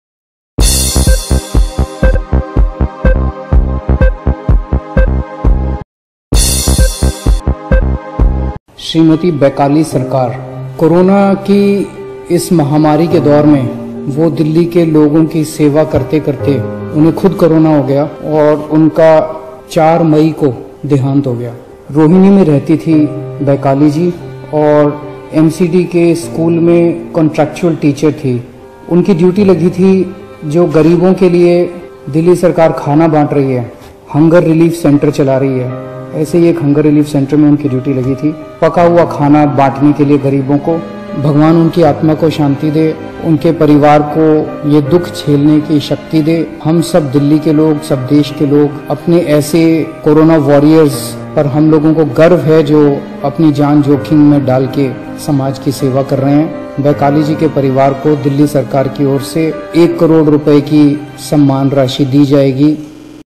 Will Provide Rs. 1 Cr As Token Of Respect To Family Of MCD Teacher Dies Of COVID-19: Arvind Kejriwal
हमारे MCD स्कूल की टीचर बैकाली सरकार को दिल्ली के लोगों की सेवा करते हुए कोरोना हो गया, जिससे उनका देहांत हो गया। बैकाली जी जैसे कोरोना वॉरियर्स पर देश को गर्व है।

उनकी जान की कोई कीमत नहीं है, लेकिन उनके परिवार की मदद के लिए दिल्ली सरकार उन्हें ₹1 करोड़ की सम्मान राशि देगी।